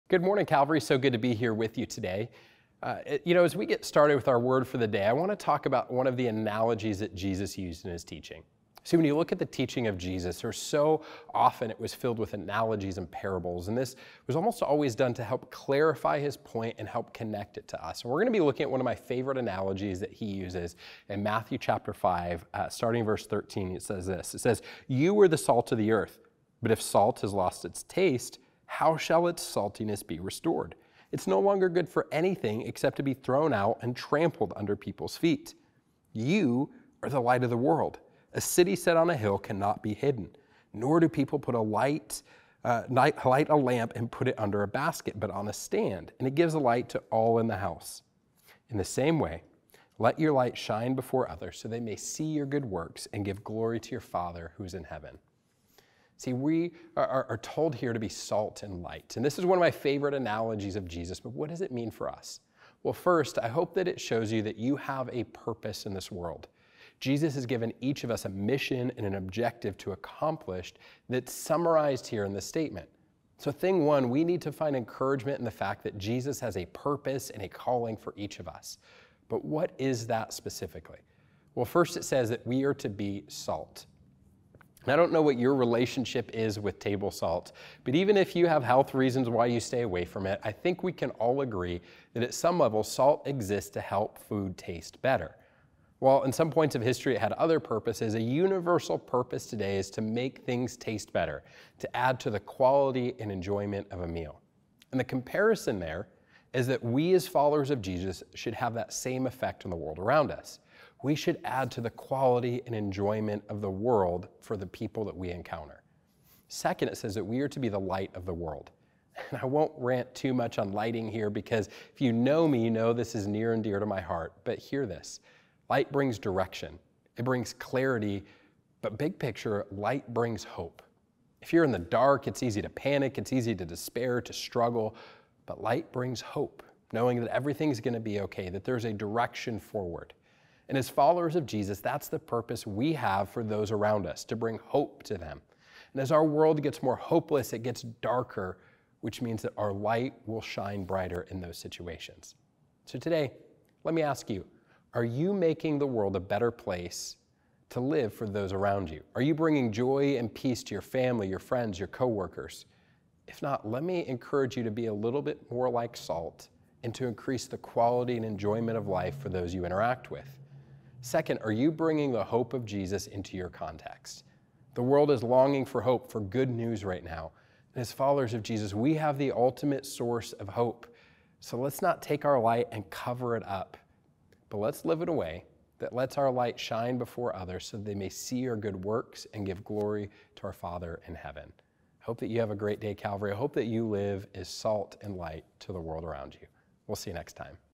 Your Word for the Day - Episode 562
Today Pastor Robert presents your purpose for life.